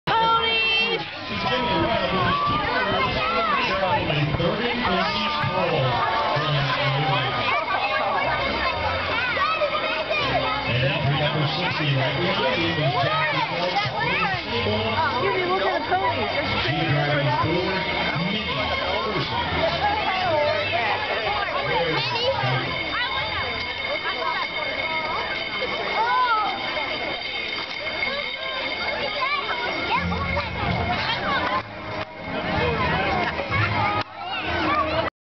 Mei Digs Tiny Horses
Mei swoons at the sight of little horses @ the 2010 Mount Orab Christmas parade.